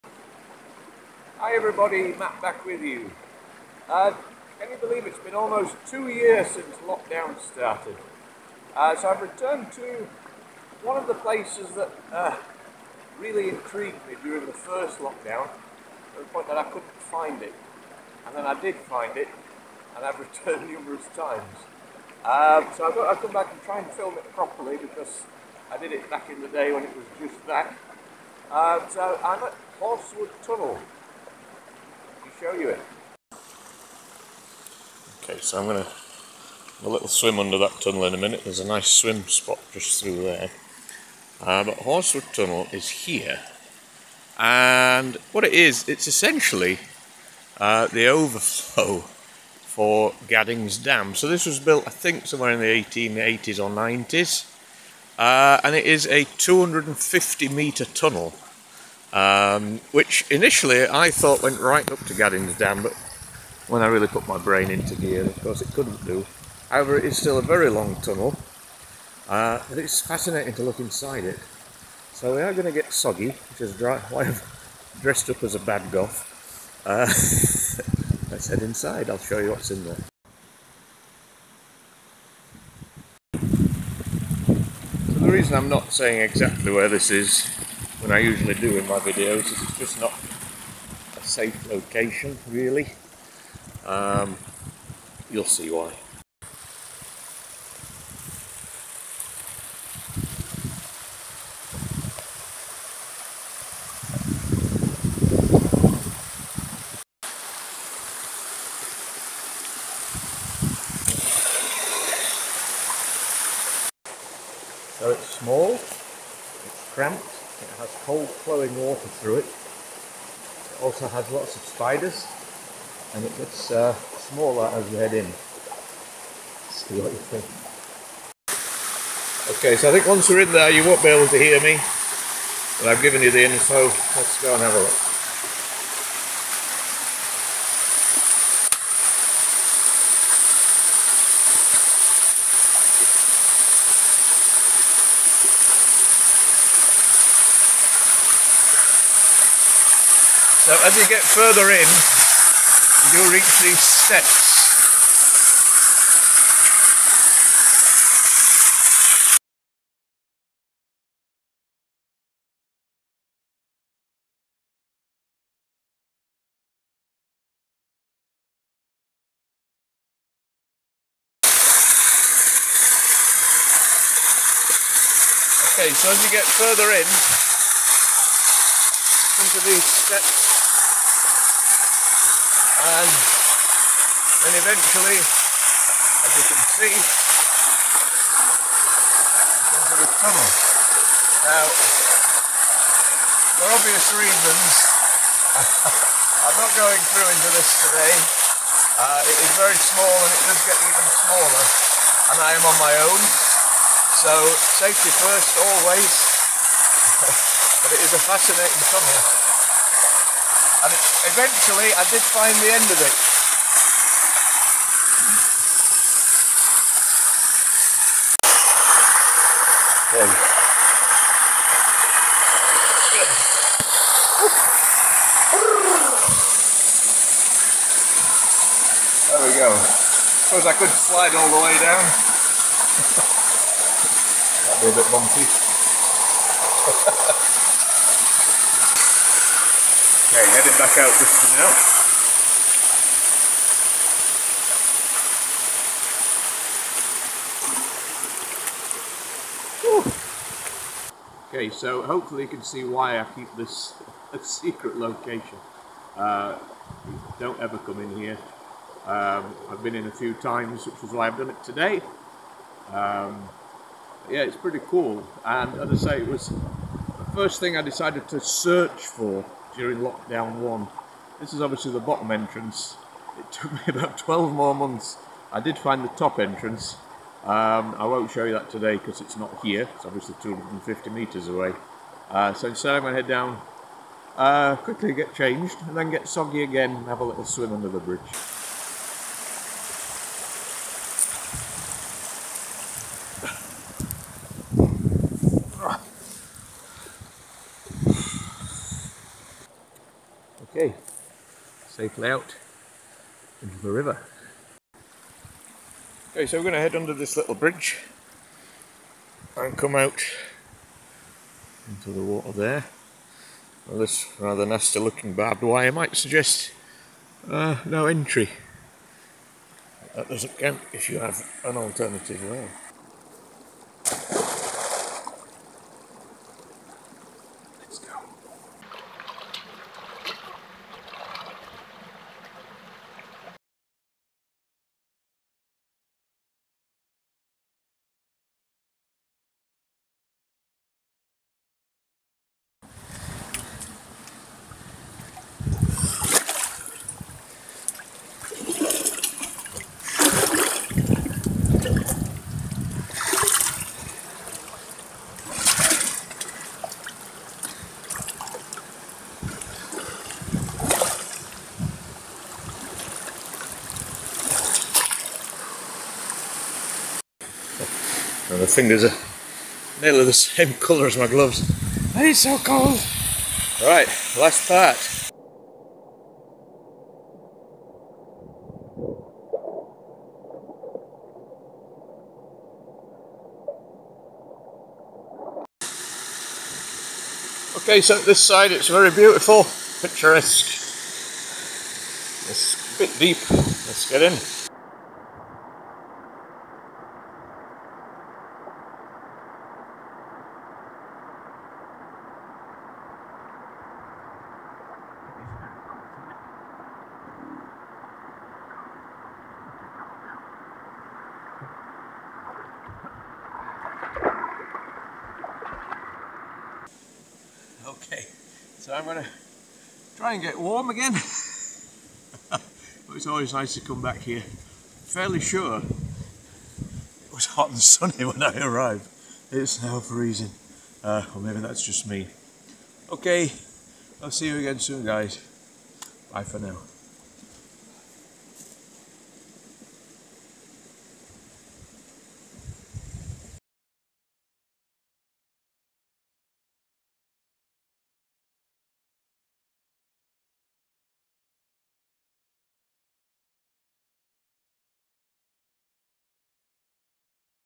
Journey into Horsewood Tunnel & Wild Swim. Lumbutts. Todmorden
Almost 2 years ago since Lockdown started I've returned to Horsewood Tunnel which, for a while, I spent the majority of Lockdown 1 searching for. The tunnel is a 250 metre underground tunnel which is officially an overflow for Gaddings Dam, built around the 1830s. (I say 1880s on the video, apologies)

Also nearby is a lovely spot for a wild swim so let's get the goth gear on and explore ✌️😜✌️